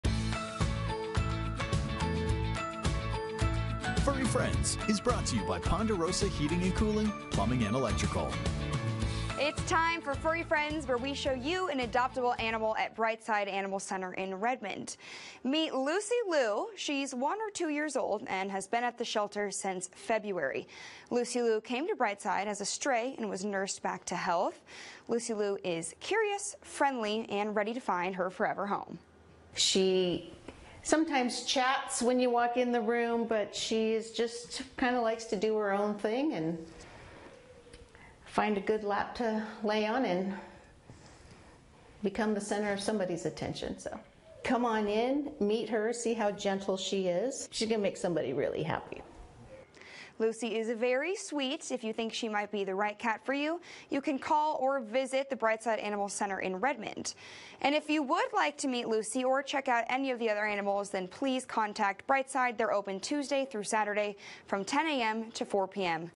Furry Friends: Lucie Liu is a curious, friendly girl
Lucie Liu is a sweet, gentle 1- or 2-year-old and has been waiting for a new, loving home since February at BrightSide Animal Center in Redmond.